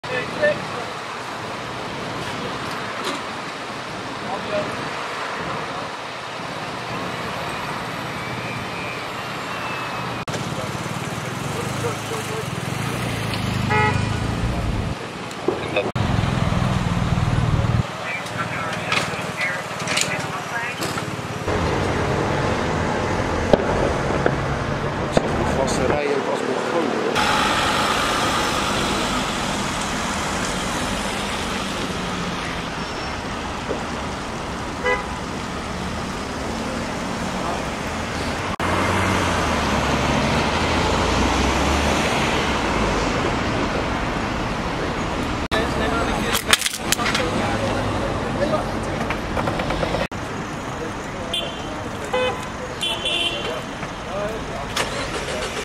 Bestuurder van bezorgscooter gewond na aanrijding met auto Troelstrakade Den Haag
Den Haag – Donderdagavond 29 december heeft er op de Troelstrakade ter hoogte van de Moerweg een aanrijding plaatsgevonden tussen een auto en een bezorgscooter. De bestuurder van de bezorgscooter raakte hierbij gewond...

Meer info en foto’s in ons item op onze website of via onze District8-app